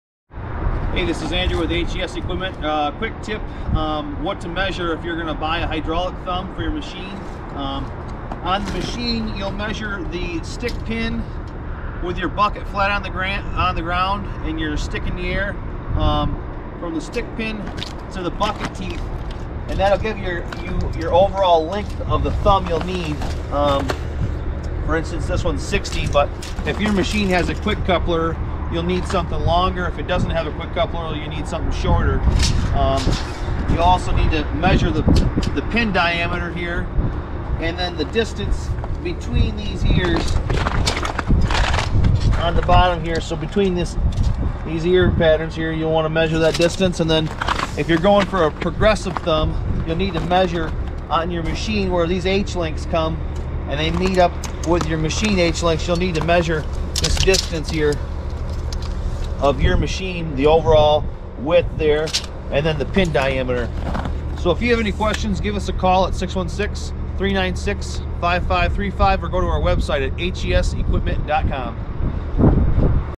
How to Measure Your Thumb with HES
Watch to see the most crucial parts of measuring a thumb when getting it to properly fit to your machine.